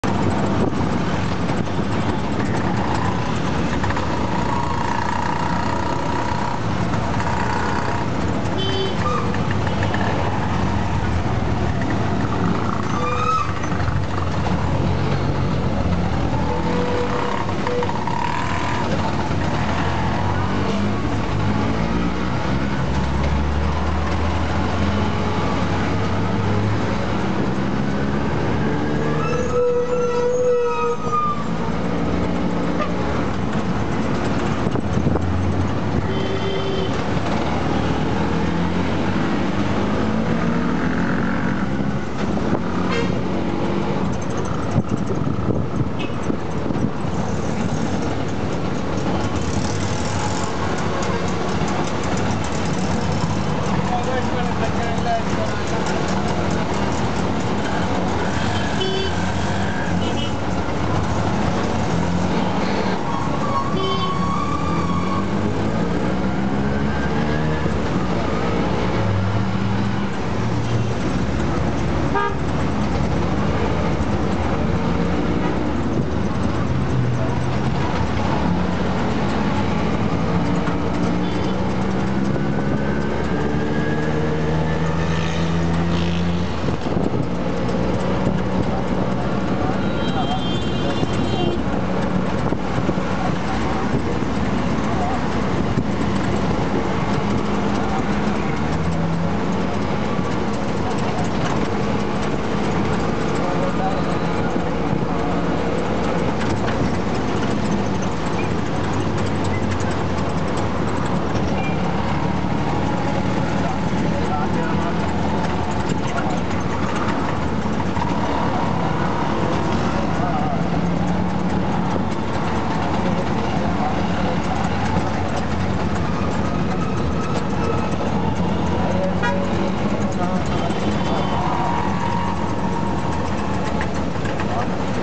Two minutes in a Sri Lankan three-wheeler/tuk-tuk
Two minutes from a trip from Mt Lavinia towards the Galle Face Hotel in a Sri Lankan three-wheeler/tuk tuk.